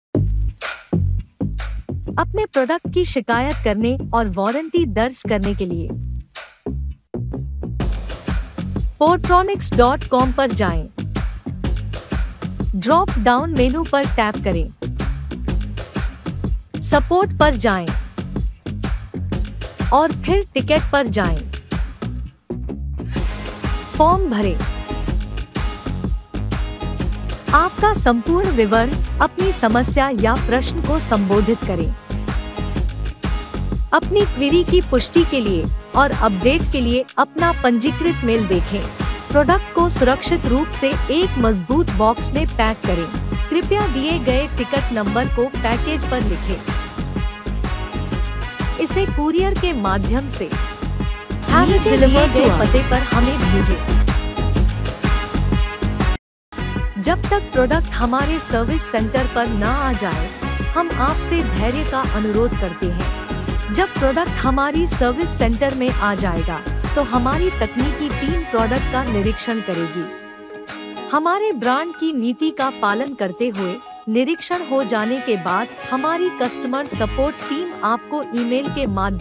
Portronics India | अपने प्रोडक्ट की शिकायत और वॉरंटी दर्ज कैसे करें
पोर्ट्रोनिक्स में हम केवल आपका प्यार चाहते हैं, यही हमें भारत का सबसे पसंदीदा ब्रांड बनाता है। हालाँकि, कभी-कभी ऐसी स्थितियाँ हो सकती हैं जब प्यार की परीक्षा हो जाती है यानी, आपके द्वारा खरीदा गया उत्पाद काम नहीं करता है या वॉरंटी अवधि से कुछ समय पहले अचानक काम करना बंद कर देता है। यह वीडियो अनुरोध करने या वॉरंटी दर्ज करने के तरीके के बारे में एक मार्गदर्शिका है। अगर ऐसा होता है कि उत्पाद के साथ कोई समस्या है तो वीडियो देखें। हमें उत्पाद भेजें, हमारी टीम इसकी जांच करेगी और फिर उत्पाद को प्रतिस्थापित या सेवा प्रदान करेगी (उत्पाद और ब्रांड की नीतियों के आधार पर) और फिर उत्पाद को आपको वापस भेज देगी।
हम हमेशा एक ग्राहक के रूप में आपके अनुभव को ऊंचा करना चाहते हैं। पोर्ट्रोनिक्स को चुनने के लिए धन्यवाद!

Be a part of our community to pamper yourself with the best lifestyle buddy, follow us on: